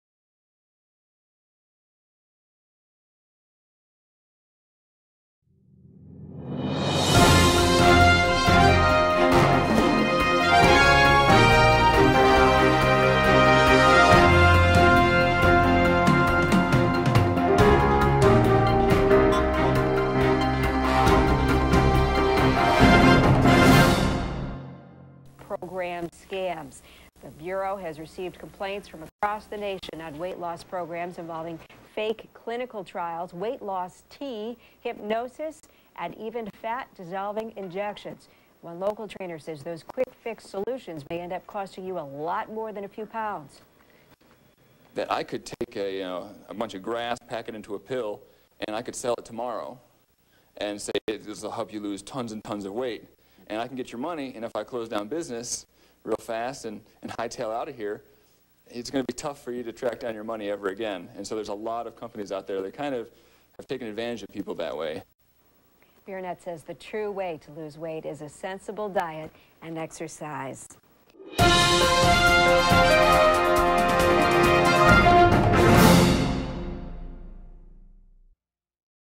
Tall Trainer Talks About Weight Loss Scams
I was interviewed on the news to talk about the recent information released by the Better Business Bureau.  There has been a 40% increase in complaints about weight loss programs and pills in the last five years.  It didn't make the final edit for TV but I also talked about how health clubs have had a 90% increase in complaints to the BBB since 2002.  Most of these complaints are about the contact or billing (about 80%).

This is just a quick report.  More of a sound bite than a full interview.